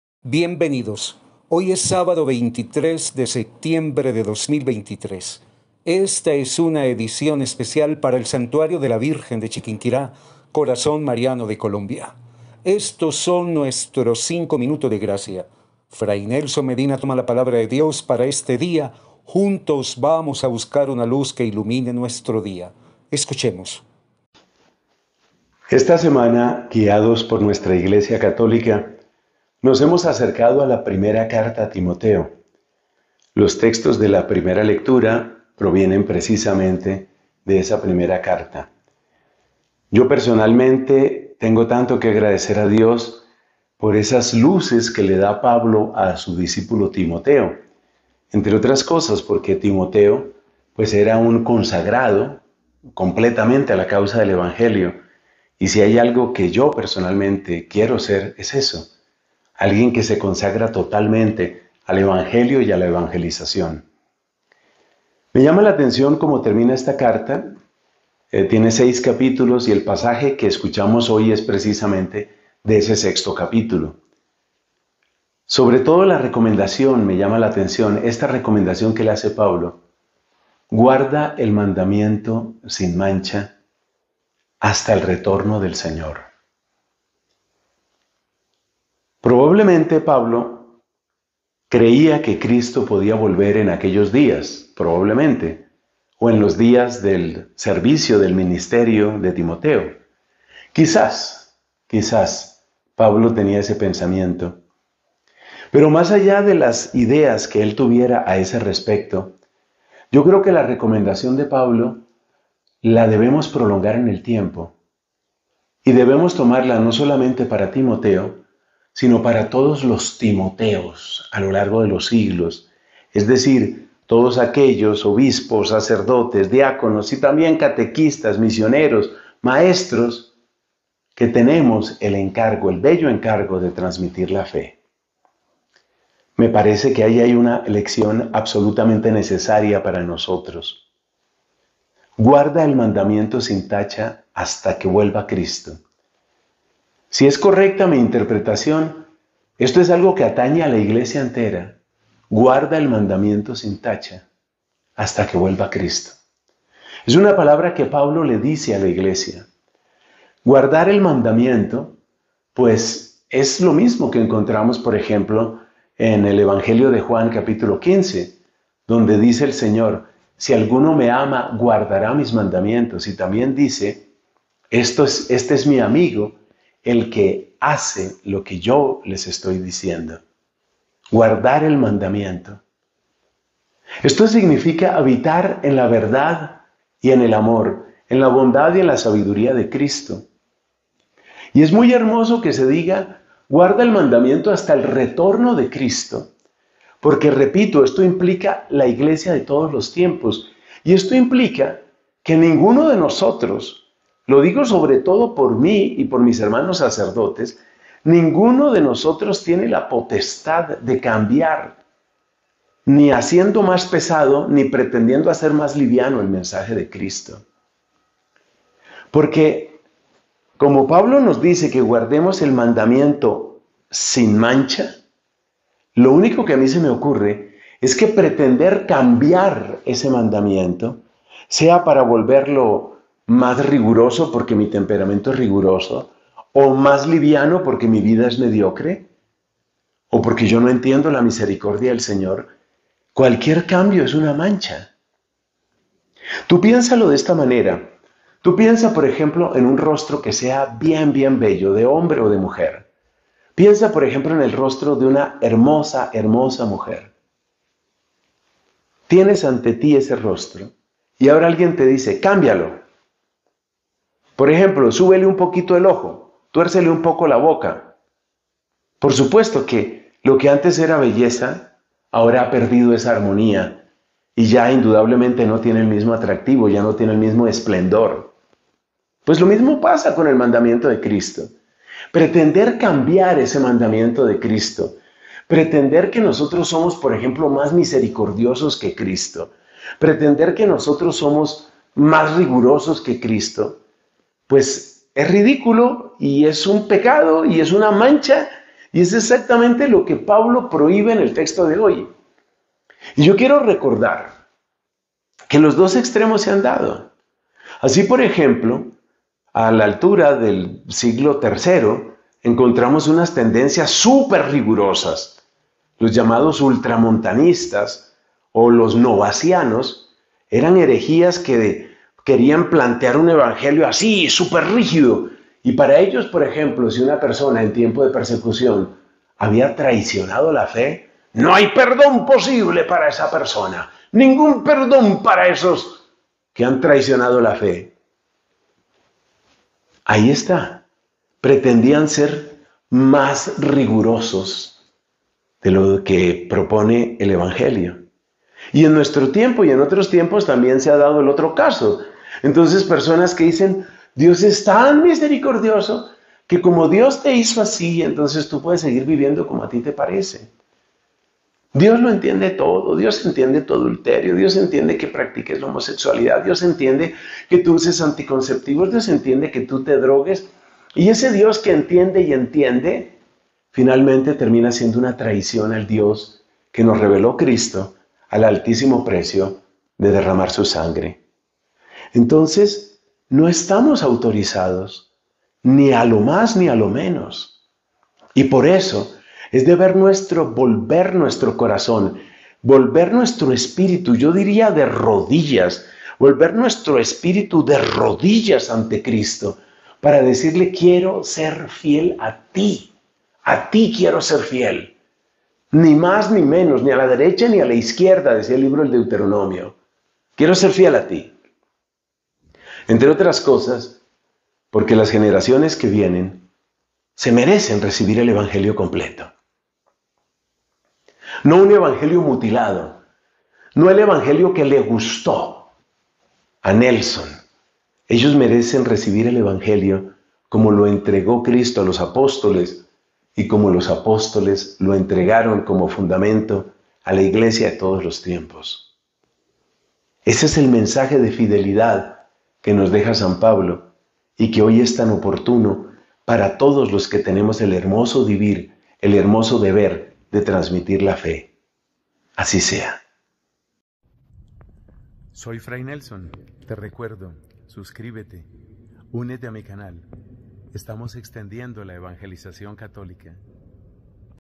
¡LA GRACIA! Sábado, 23 de Septiembre de 2023
Pretender cambiar el mandamiento de Jesús, creer que somos más misericordiosos o más rigurosos que Él es ridículo y es pecado. * * *